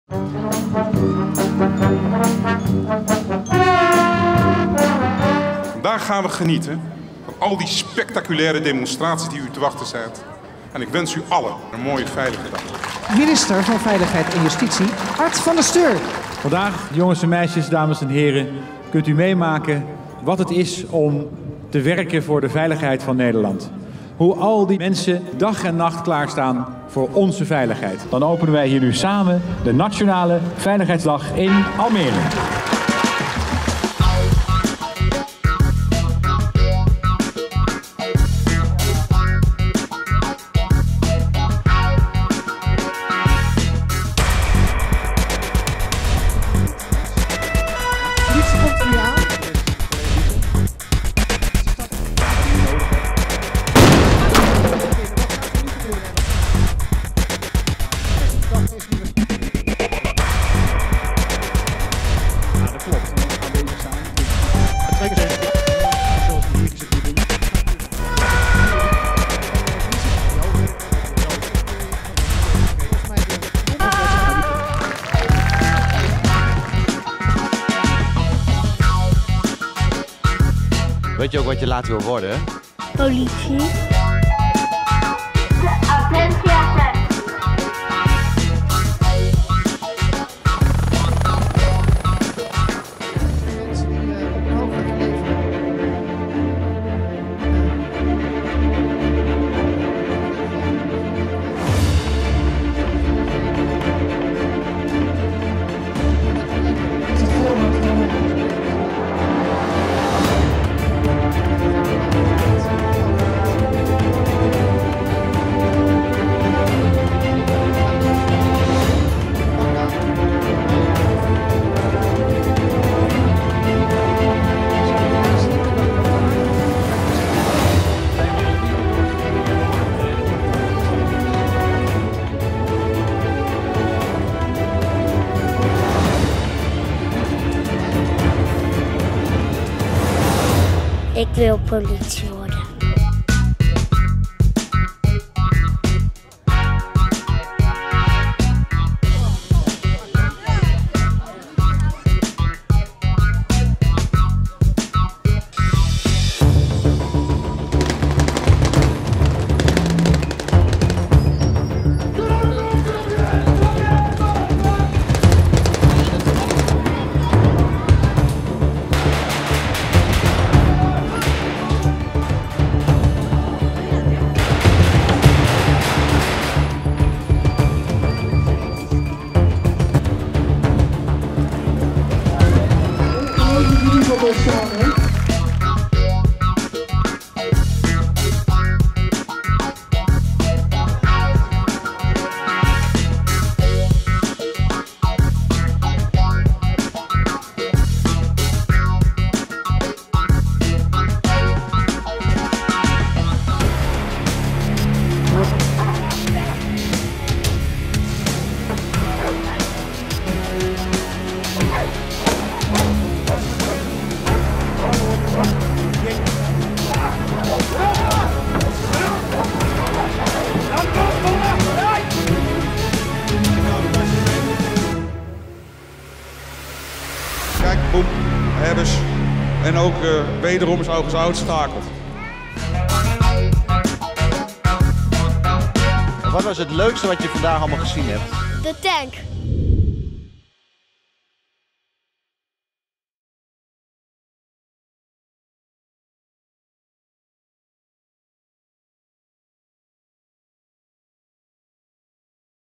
After Movie Landelijke Veiligheidsdag 2016
De Landelijke Veiligheidsdag vond in 2016 voor de derde keer plaats in het centrum van Almere. Met een ferme druk op de knop openden burgemeester Franc Weerwind en minister Ard van der Steur van Veiligheid en Justitie officieel de Landelijke Veiligheidsdag. Direct daarna startte de eerste demonstratie, waarbij de politie twee gewapende overvallers middels het arrestatieteam arresteerde, de ambulancemedewerkers een van hen afvoerde naar het ziekenhuis en de brandweer een daaropvolgende brand bluste. Het evenement trok opnieuw veel belangstellenden naar de het centrum van Almere. 45.000 mensen bezochten het  evenement. Op de Esplanade, het Forum en het Stationsplein was te zien wat het werk van politie, brandweer, defensie, ambulance, KNRM, kustwacht en reddingsbrigade inhoudt en mochten kinderen op voertuigen zitten of ze van binnen bekijken.

Elke jaar breidt de Landelijke Veiligheidsdag uit met verschillende onderdelen. Een van de nieuwe demonstraties was het werk van USAR. Deze landelijke organisatie werkt nauw samen om slachtoffers te redden bij grote rampen in binnen- en buitenland. Specialisten van politie en ambulancediensten uit vier veiligheidsregio’s werken nauw samen met defensie en Medisch Centrum Haaglanden om slachtoffers onder het puin vandaan te halen. In Almere liet deze speciale dienst zien hoe dat in zijn werk gaan.
Op het water werd een reddingsactie nagebootst als er in Nederland dijken doorbreken en bewoners naar het dak van hun huis moeten vluchten. Wie wordt er als eerste gered en wie komt later aan de beurt en hoe werken de verschillende hulporganisaties als Reddingsbrigade Nederland en KNRM samen. Ook werd bij de hulpactie in Almere een helikopter ingezet om mensen in nood van de daken te halen en naar veiliger gebieden te brengen.
De Landelijke Veiligheidsdag werd voor de derde keer in Almere gehouden. Het evenement wordt georganiseerd om het werk van hulpverleners, die 24 uur per dag en zeven dagen in de week paraat staan, onder de aandacht te brengen. Ook wordt getoond hoe je zelf een bijdrage kunt leveren om in huiselijke kring de veiligheid te vergroten.